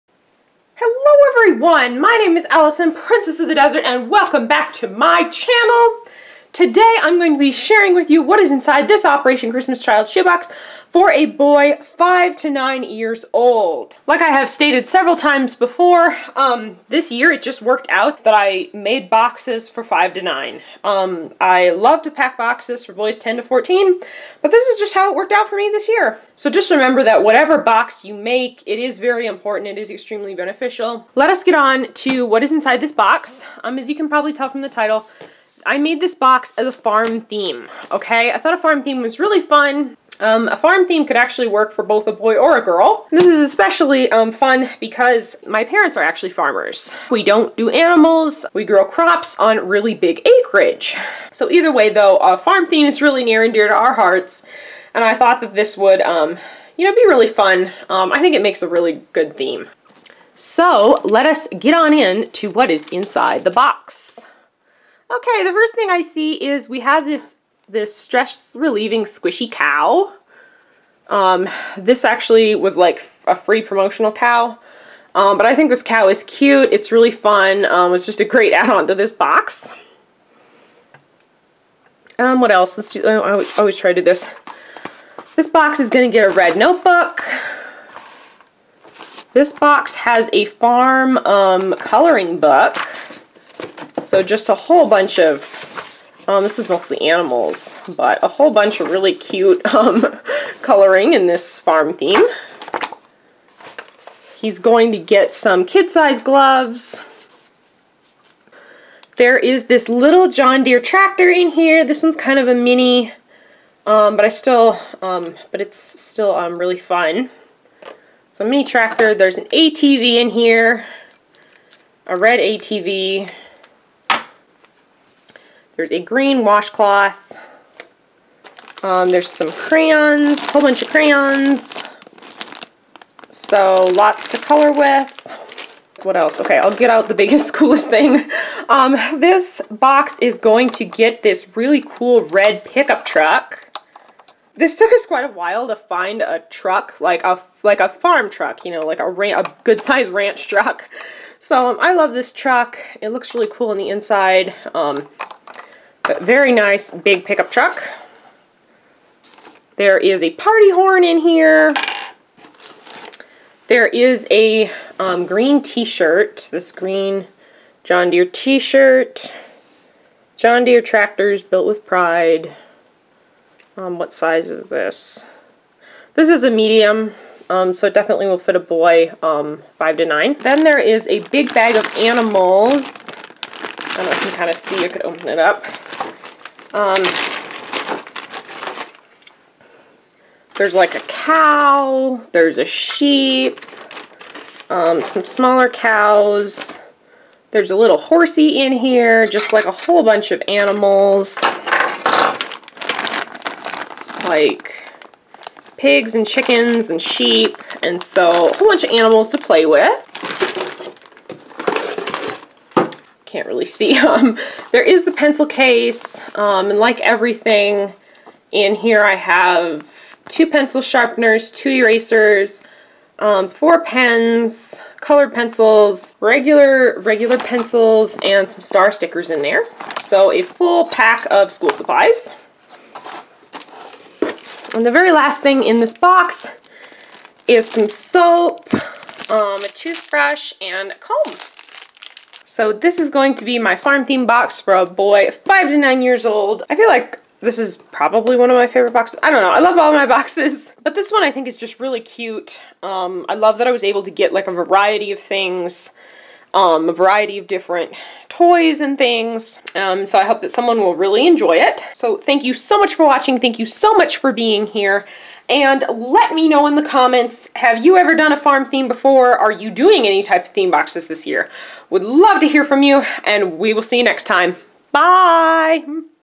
Operation Christmas Child Farm Theme Boy 5-9
Today I am super excited because I bring to you a very special Operation Christmas Child Shoebox for 2019! Today we are going to be unboxing a Farm theme box for a boy 5-9 years old.

What is in the Box:

Things to Play With:
Squishy Cow
Big Red Ford Pick Up Truck
John Deere Tractor
Red ATV
Bag of all kinds of different plastic Farm Animals
Party Horn

Things to Learn With:
Red Notebook
Farm Animals Coloring Book
Homemade Crayons
Colored Pencils
Pencils
2 Sharpeners
2 Pink Erasers
Pens
Star Stickers
Black Pencil Case

Things to Keep Yourself Clean:
Green Washcloth
Bar of Soap
Kid size Toothbrush
Red comb

Things to Wear:
John Deere Tractors T Shirt (Size Medium)
Kid Size Gardening / Work Gloves

I will also include a photo of myself, a little note of encouragement to the child, along with $9 to cover shipping costs, so the box can travel to its destination.